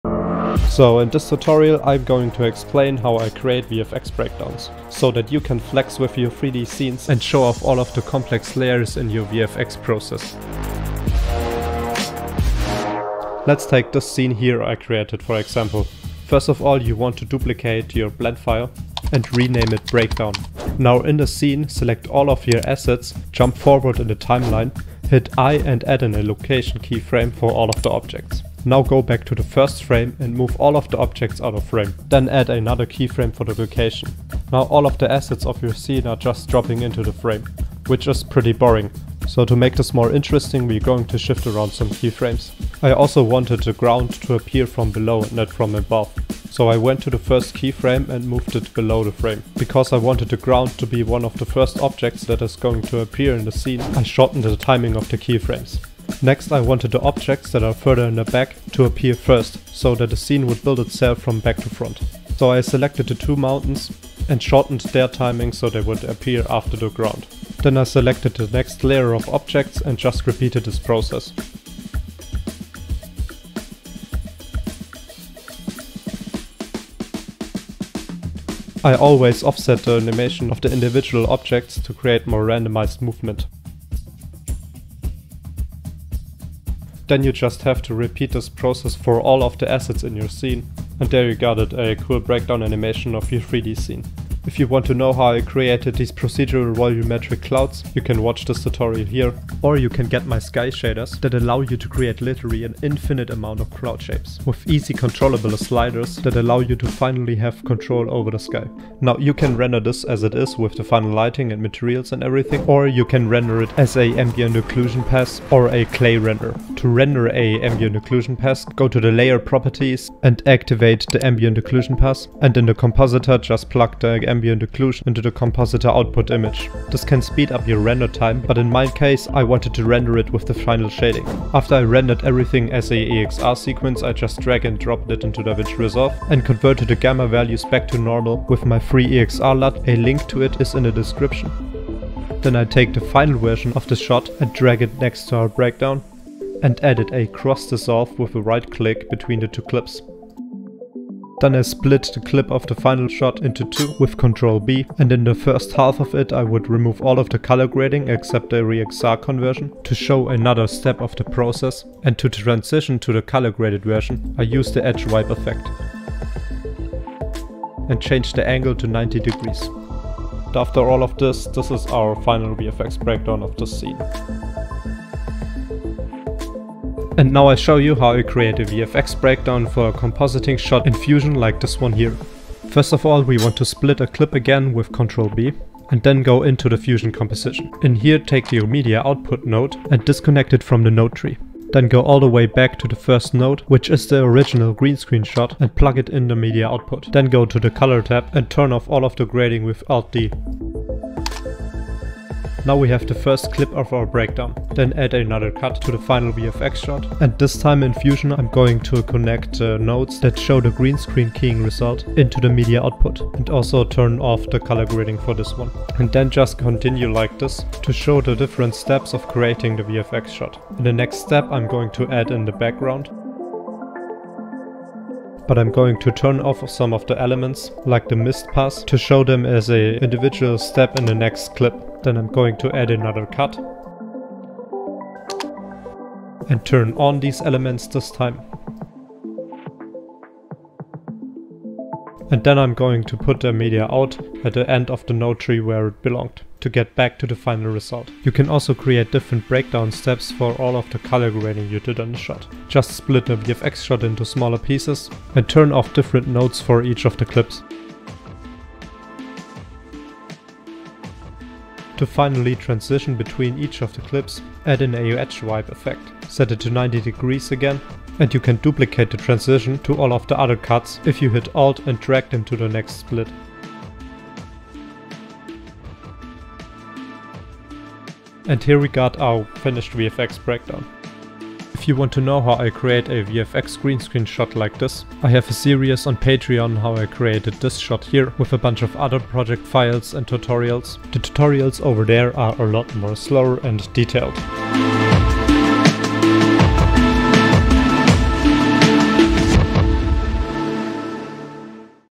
How to create amazing VFX Breakdowns | Blender | Resolve
In this video, I explain how I create VFX Breakdowns to show off the VFX process in an eye-catching way.